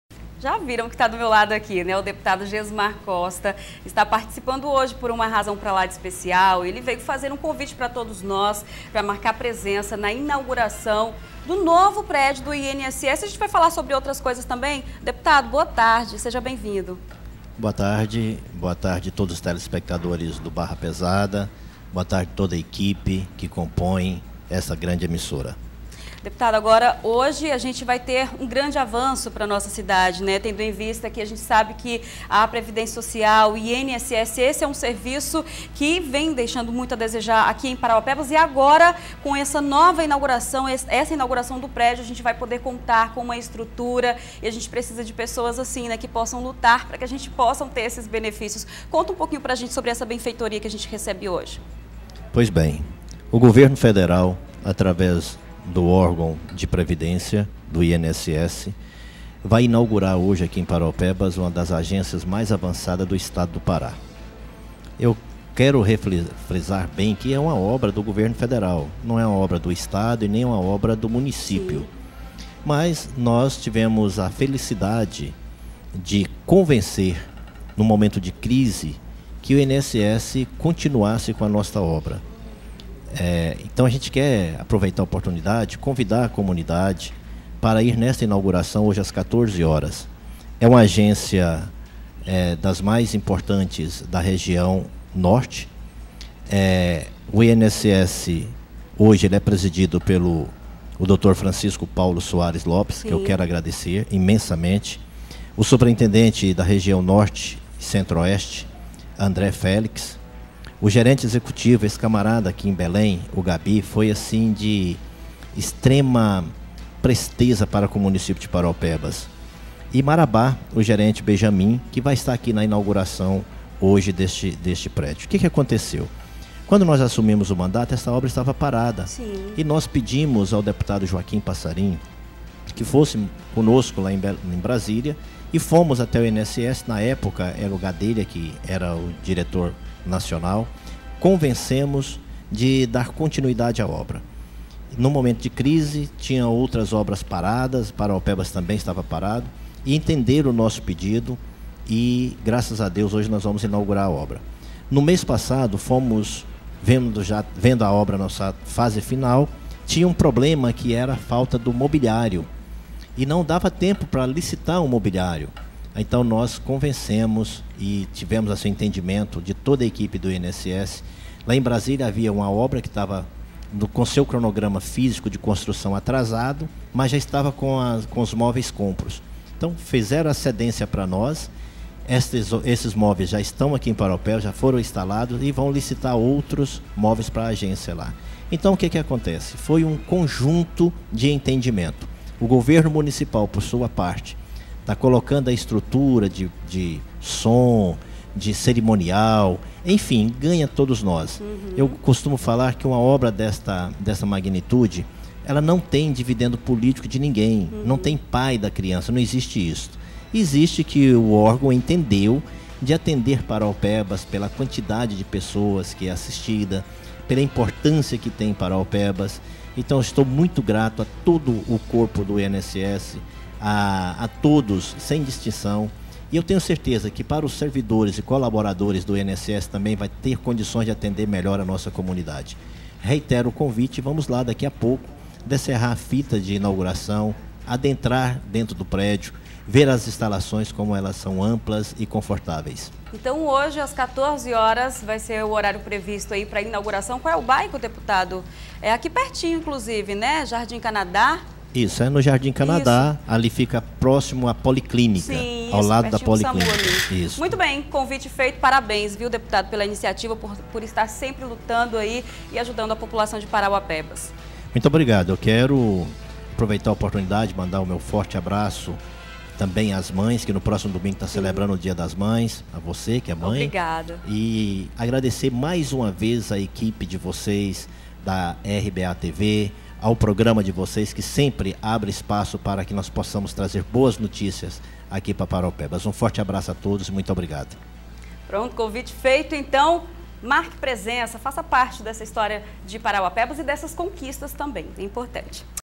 CONVITE: INAUGURAÇÃO DO NOVO PRÉDIO DO INSS
Entrevistado: Gesmar Costa - Deputado Estadual